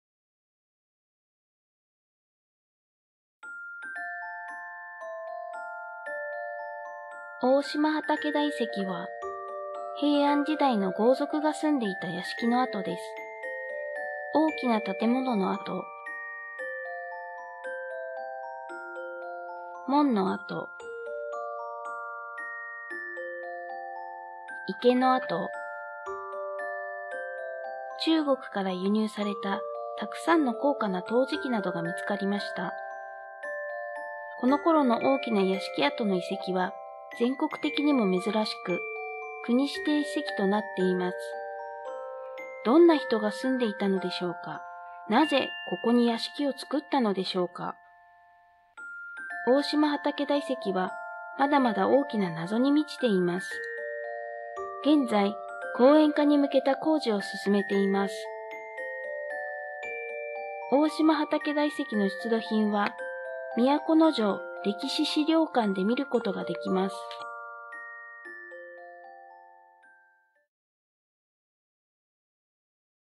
ぼんちくんと歴史探検「大島畠田遺跡」(都城市文化財)
「広報都城11号」に掲載された「ぼんちくんと歴史探検・大島畠田遺跡」の動画版­です。

大島畠田遺跡は平安時代の豪族の屋敷跡です。大きな建物とその横に掘られた池、門や道などといった、屋敷の全体像がわかる遺跡であり、国指定史跡となっています。